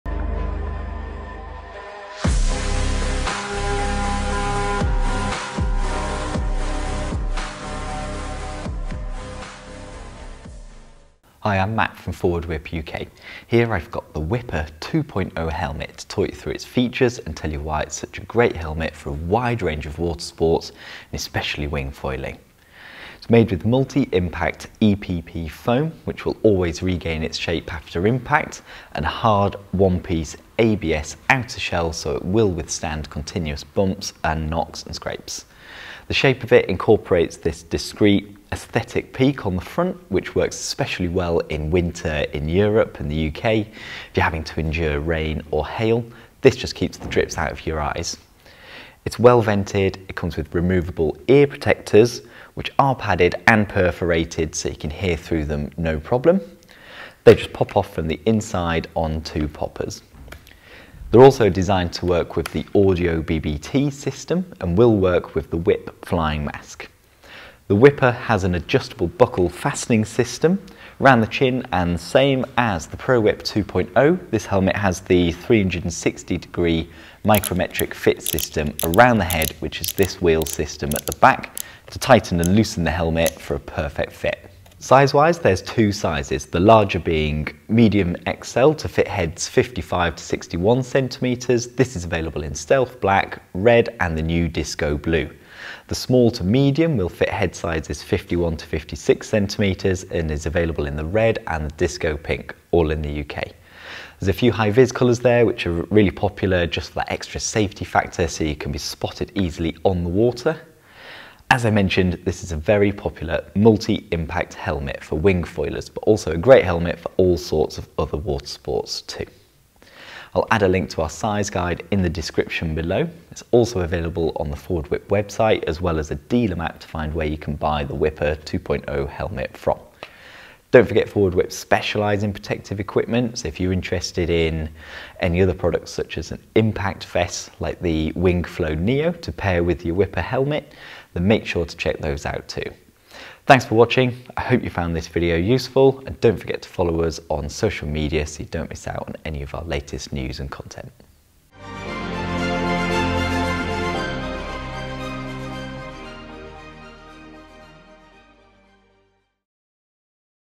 Forward WIP Wipper 2.0 Helmet
Matt from Forward WIP UK talks you through the Wipper 2.0 Helmet covering  the features, why it’s such a great helmet for a wide range of watersports and what's available in the UK.

Highlight - This is the No.1 Helmet we would recommend for Wingfoiling!
RRP £69.99